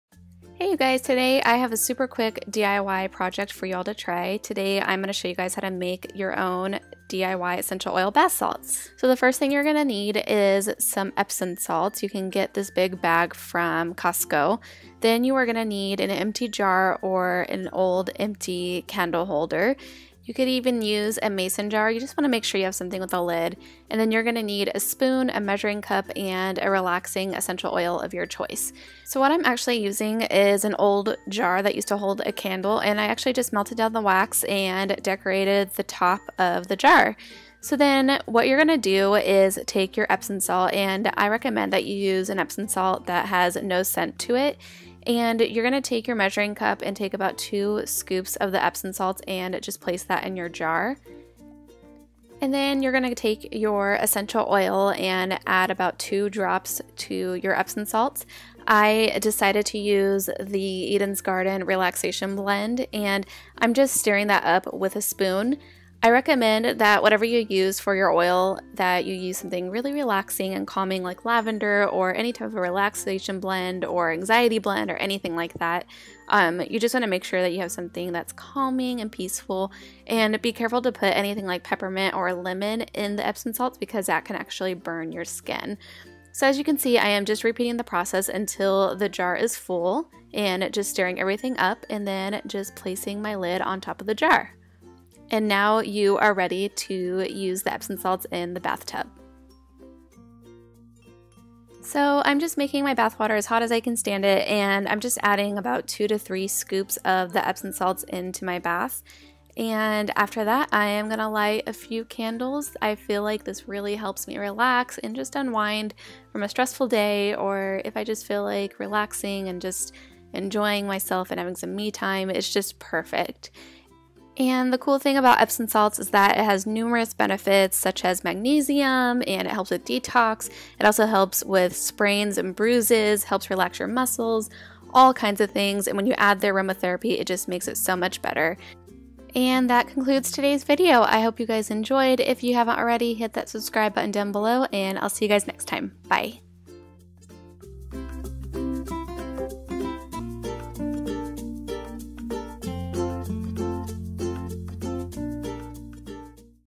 ESSENTIAL OIL BATH SALTS | DIY
(WATCH IN HD!) I hope you guys enjoyed video! This DIY is seriously so easy and worth doing!

Epsom Salts are from Costco, but they also sell this brand at Target/Walmart etc.